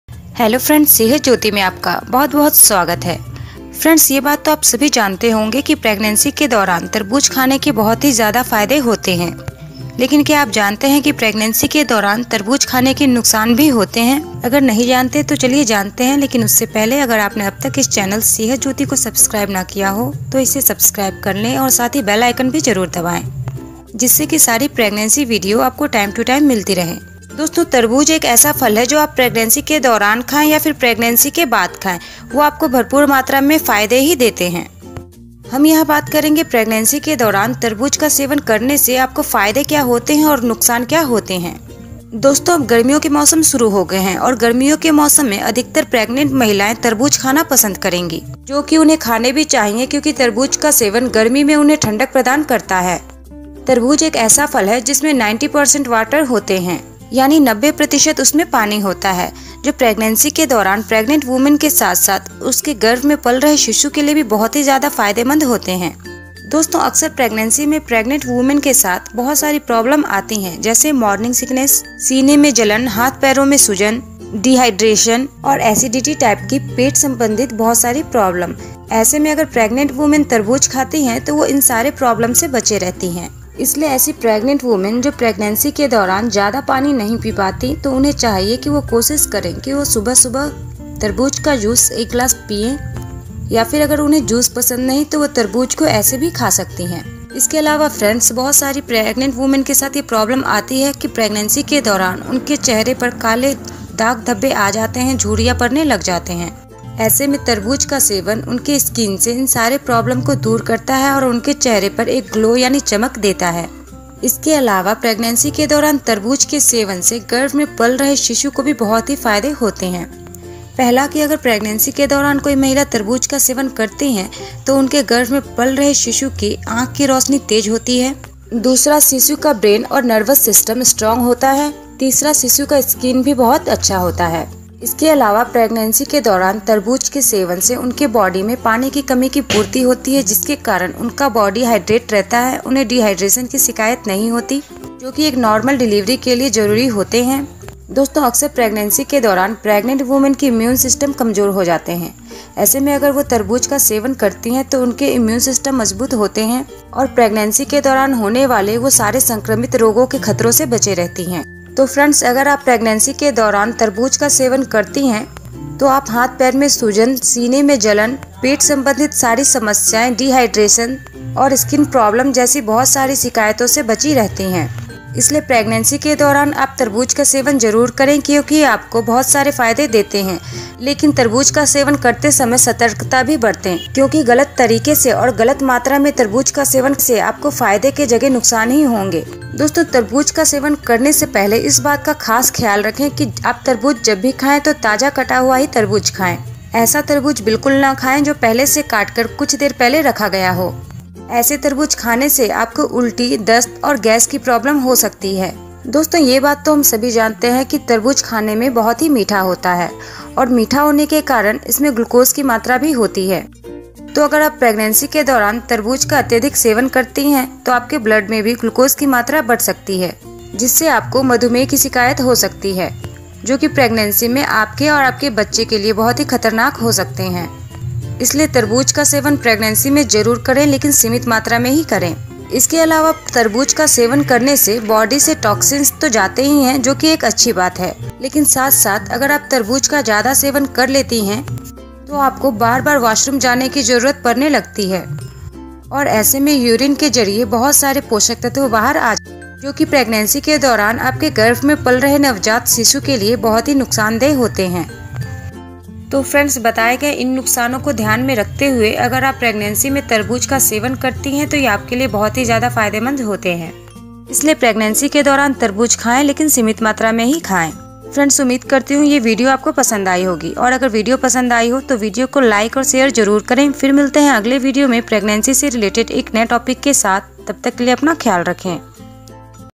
प्रेगनेंसी में तरबूज खाने के फायदे और नुकसान |water melon during pregnancy
friends iss video me maine bataya hai pregnancy ke dauran watermelon yani tarbuj khane ke fayede or nukshan ke bare me agar apko meri video pasand aaye to video ko like or share jarur karen.

Thanks